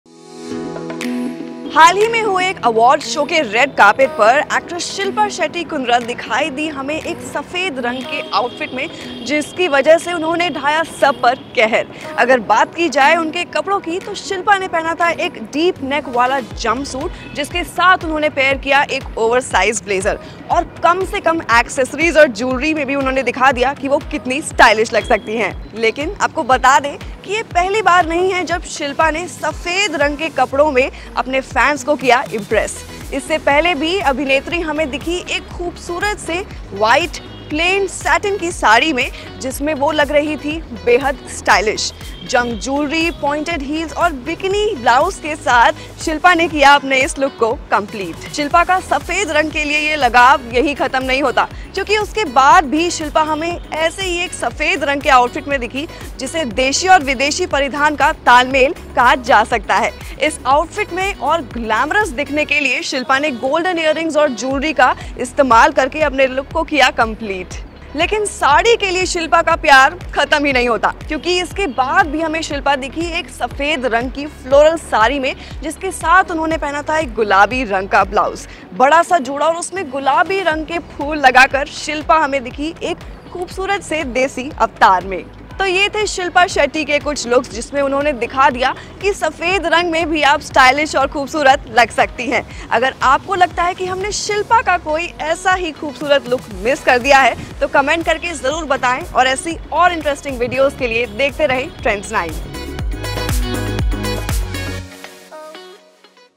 Shilpa Shetty और उनके सफ़ेद लिबास!
एक्ट्रेस shilpa shetty kundra हाल ही में एक अवॉर्ड शो के रेड कार्पेट पर व्हाइट कलर के आउटफिट में नजर आईं। हालांकि, हमने देखा कि अभिनेत्री को सफेद रंग बहुत पसंद है और कई बार उन्हें इसी रंग के आउटफिट पहने हुए देखा गया है। अधिक जानने के लिए वीडियो देखें!